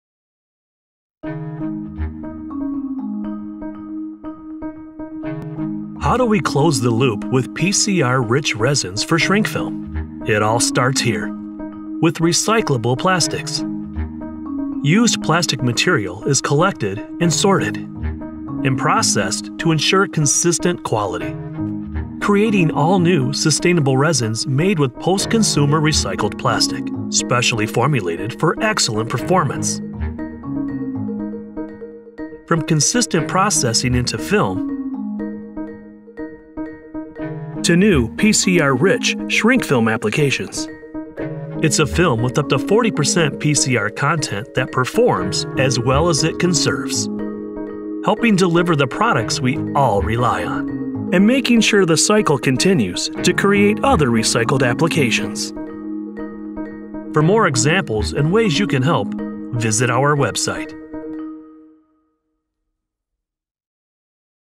PCR solution for shrink film in North America
Made with up to 70% recycled material, Dow’s new sustainable post-consumer recycled (PCR) rich resin helps brands and organizations in North America achieve their sustainability goals by offering the ability to create shrink film with up to 40% post-consumer recycled content.